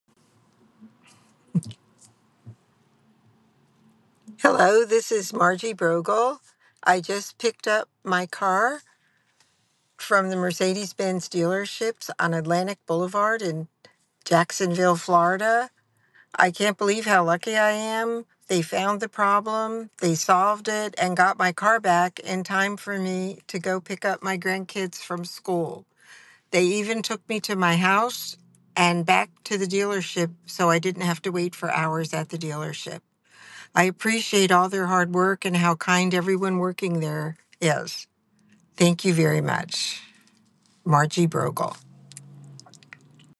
Margaret Brogle's 5-Star Experience at Mercedes-Benz of Jacksonville
Margaret Brogle shares feedback from her experience with our Team at MercedesBenz of Jacksonville. Margaret says of the service she received "I appreciate all their hard work and how kind everyone working there is..." We're here to exceed your expectations!